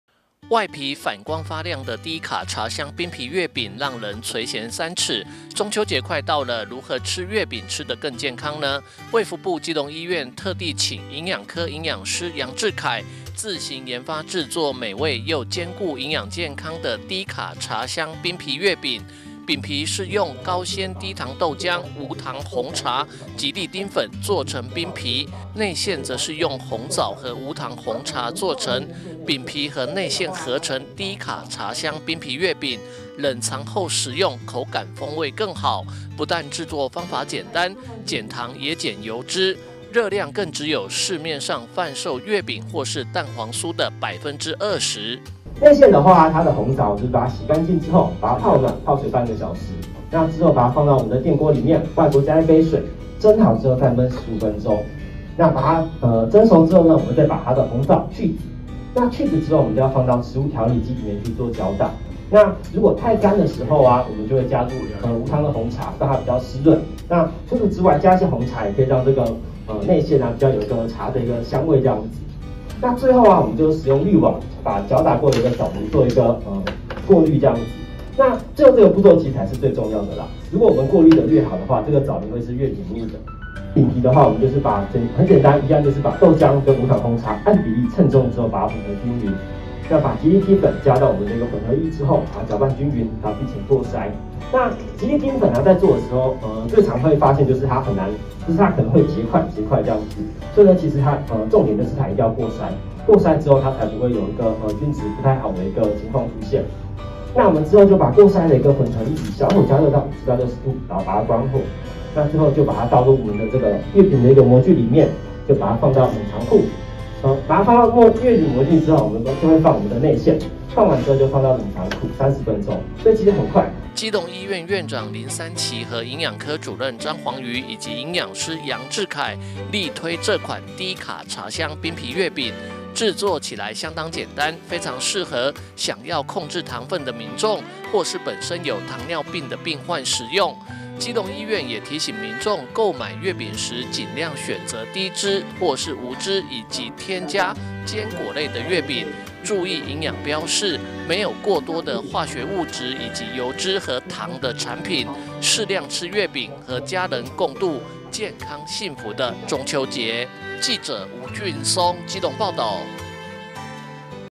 基隆|林三齊|衛福部基隆醫院|部基|中秋節|月餅|營養師|低卡茶香冰皮月餅|熱量|油脂|減糖|無糖|紅茶|吉力丁粉|紅棗|部基「低卡茶香冰皮月餅」熱量僅市售月餅20%
部基「低卡茶香冰皮月餅」熱量僅市售月餅20%

(中嘉吉隆新聞記者吳俊松/基隆報導)中秋節快到了，如何吃月餅吃得更健康呢？衛福部基隆醫院特地請營養師自行研發製作美味又兼顧營養健康的「低卡茶香冰皮月餅」，不但製作方法簡單，減糖也減油脂，熱量更只有市面上販售月餅或是蛋黃酥的百分之二十！

外皮反光發亮的低卡茶香冰皮月餅，讓人垂涎三尺，中秋節快到了，如何吃月餅吃得更健康呢？衛福部基隆醫院特地請營養科營養師楊智凱，自行研發製作美味又兼顧營養健康的「低卡茶香冰皮月餅」，餅皮是用高纖低糖豆漿、無糖紅茶、吉利丁粉做成冰皮；內餡則是用紅棗和無糖紅茶做成，餅皮和內餡合成「低卡茶香冰皮月餅」，冷藏後食用口感風味更好，不但製作方法簡單，減糖也減油脂，熱量更只有市面上販售月餅或是蛋黃酥的百分之二十！

基隆醫院院長林三齊和營養科主任張皇瑜以及營養師楊智凱力推這款「低卡茶香冰皮月餅」，製作起來相當簡單，非常適合想要控制糖分的民眾或是本身有糖尿病的病患食用；基隆醫院也提醒民眾，購買月餅時，盡量選擇低脂或是無脂以及添加堅果類的月餅，注意營養標示，沒有過多化學物質以及油脂和糖的產品，適量吃月餅，和家人共度健康幸福的中秋節！